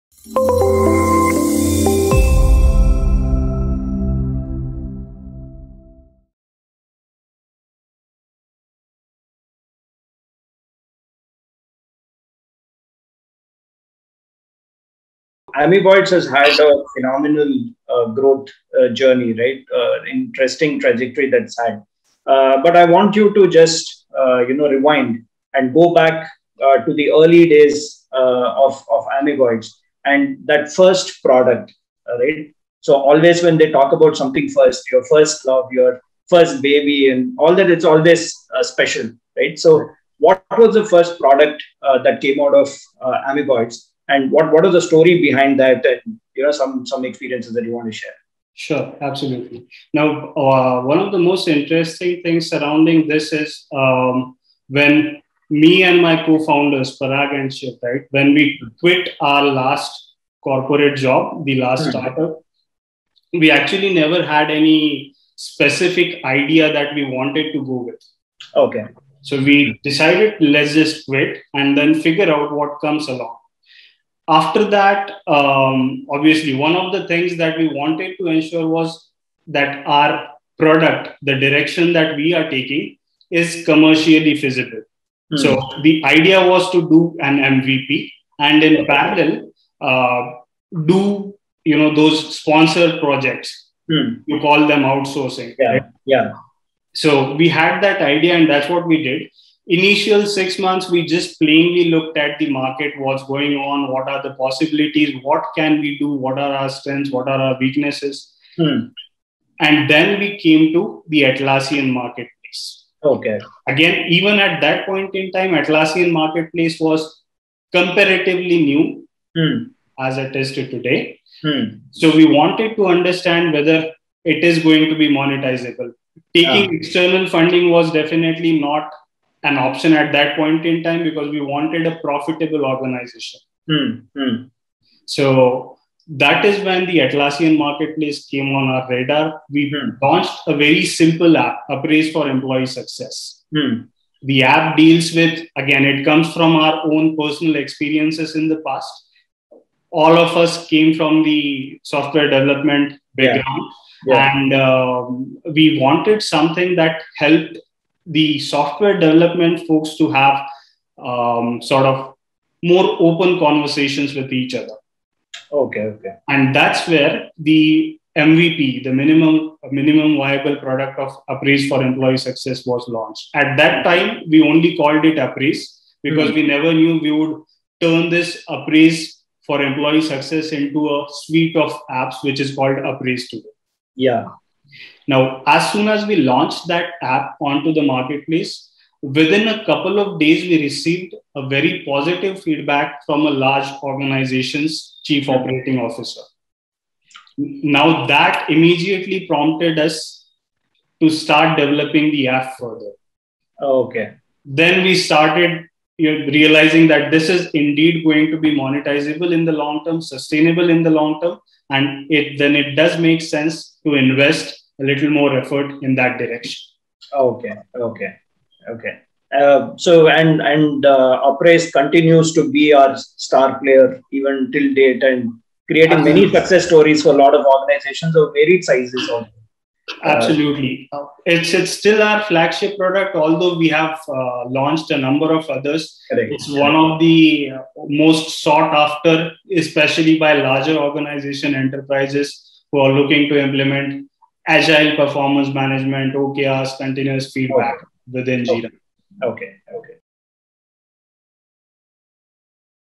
Can you rewind & go back to early Amoeboids days?-Origin Stories - Amoeboids
Starting a new business requires a special set of skills and attributes. However, it is individual characteristics that define success. Here is a video which takes you through the early days of how Amoeboids' started as our CEO, Anand Inamdar shares some insights.